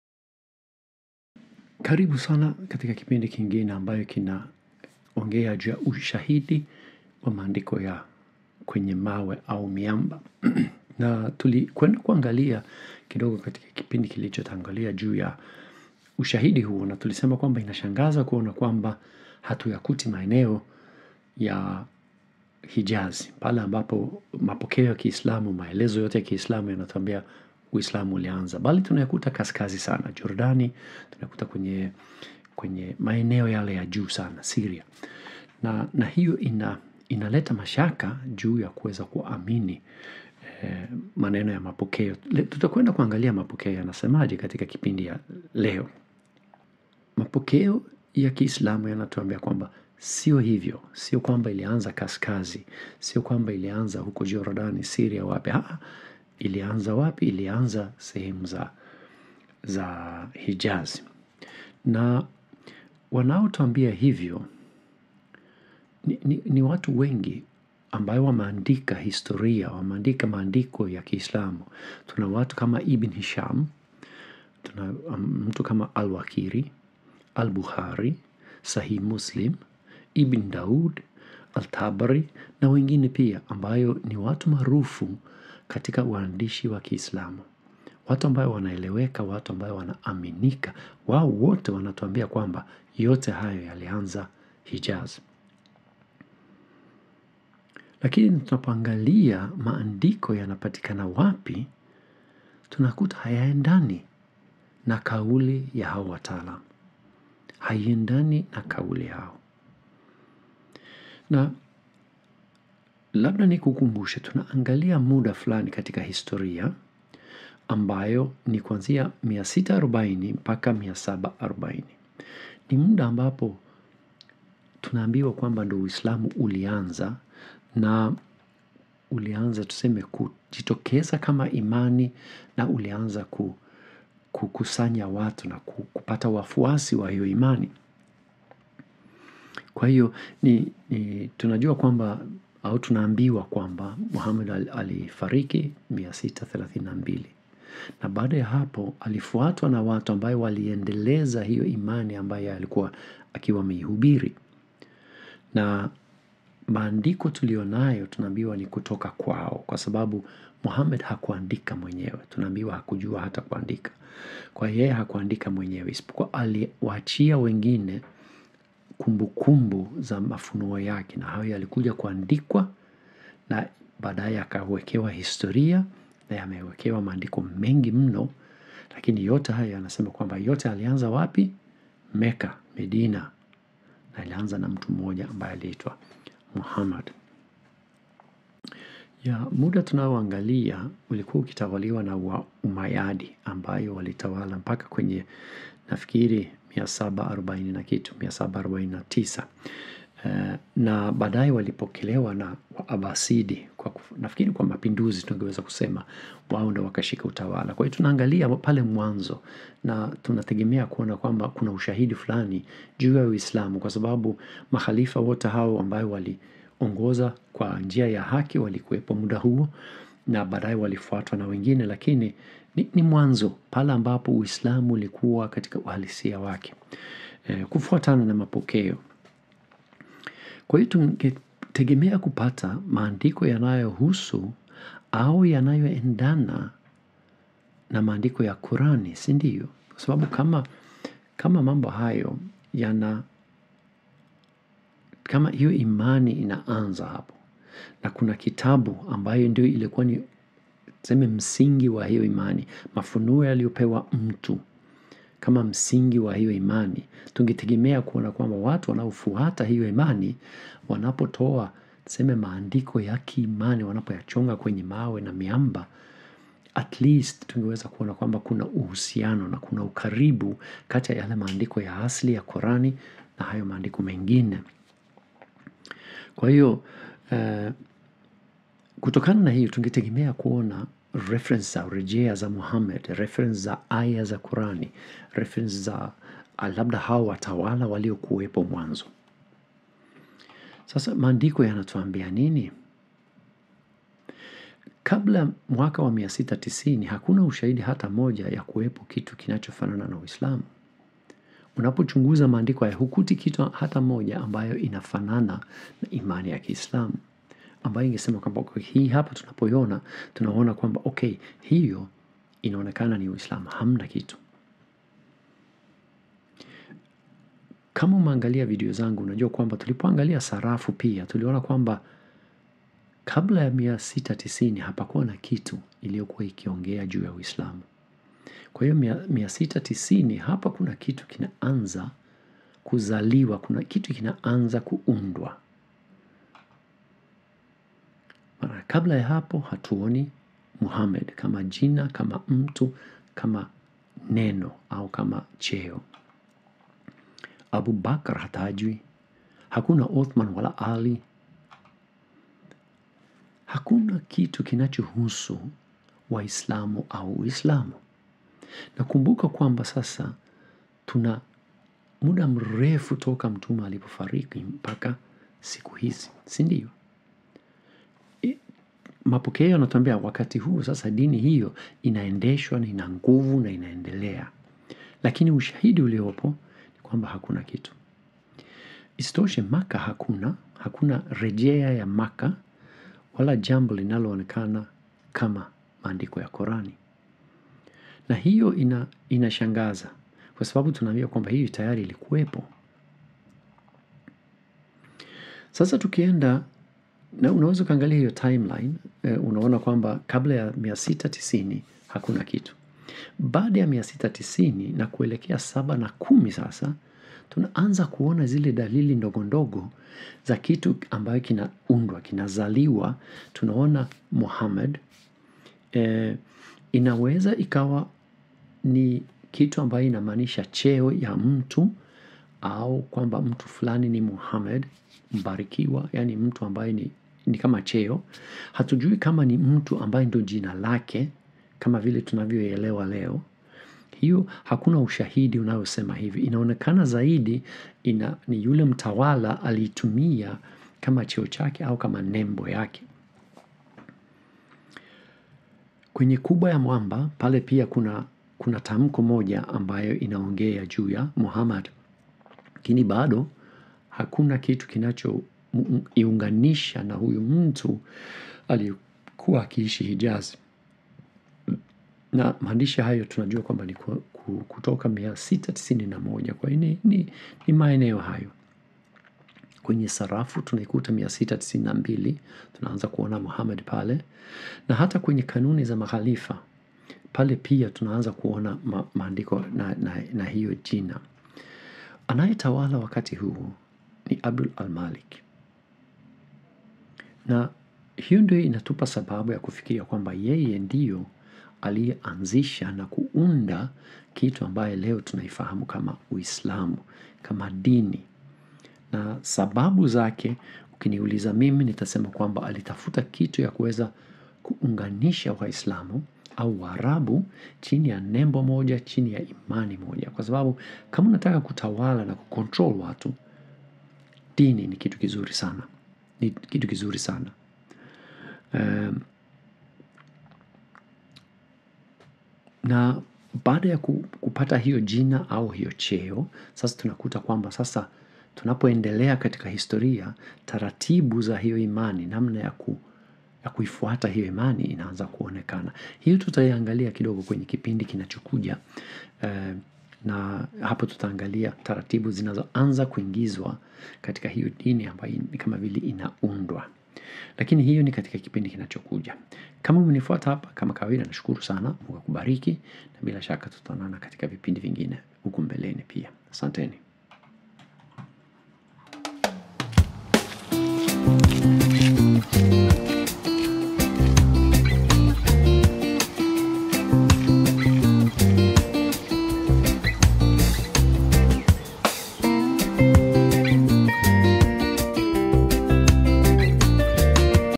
Ukosefu wa Muhammad mwanzo wa Uislamu
Kipindi cha leo;
Mapokeo ya Kiislamu yanatuambia siyo hivyo…
Hayo maandiko yanatuambia nini?
Hadi 690
690-710

Mapokeo ya Kiislamu
Kinyume na ushahidi waandishi wa Kiislamu wantuambia kwamba yote hayo yalifanyika Makka na Madina
Ibn Hisham
Al Wakiri
Al Bukhari
Sahii Muslim
Ibn Daudi
Al Tabari
Na wengine
Lakini tukiangalia maandiko yanapatikana wapi tunakuta haiendani na kauli ya mapokeo.
Tunaangalia muda wa kutoka 640 hadi 740 maana ni muda wa baada ya kifo cha Muhammad na mwanzo wa uislamu chini ya makhalifa wa haki (kufuatana na mapokeo) 624 hadi 749
Ni wakati wa Ummayadi wanatawala baada ya 740 wa Abbasidi wana fanya mapinduzi na kushika utawala.
Tunategemea kupata maandiko yanayohusu au yanayoendana na maandiko ya Qurani maana kwa jinsi tunavyoelezwa muda huo ni mwanzo wa Uislamu baada ya Mtume kuwepo.

Maandiko yanatuambia nini?
Basi kabla ya mwaka wa 690 hakuna ushahidi hata moja ya kuwepo kwa kitu kinacho fanana na Uislamu.
Tulipochunguza sarafu pia tuliona kwamba hapakuwep ushahidi ya uislamu kabla ya 690…
Hakuna Muhammad
Hakuna Abu Bakr
Hakuna Othman wala Ali
Hakuna kitu kuhusu Waislamu au Uislamu
Makka hakuna na wala jambo linaloendana na maandiko ya Qurani!
Baada ya 690 na kuelekea 710 JIna la Muhammad inaanza kuonekana na inaunganishwa na cheo.
Hiyo pia tunaikuta kwenye kuba ya mwamba na maandishi yake ya kutoka 691.
Kwenye sarafu 692
Na kanuni za makhalifa wakati huo
Na anayotawala wakati huo ni Abd Al Malik

Tumeanza kuona Muhammed kama jina au cheo lakini bado taratibu zinazoendana na Imani kama maombi nk hiyotutaangalia ilianza kuonekana lini katika kipindi kinachokuja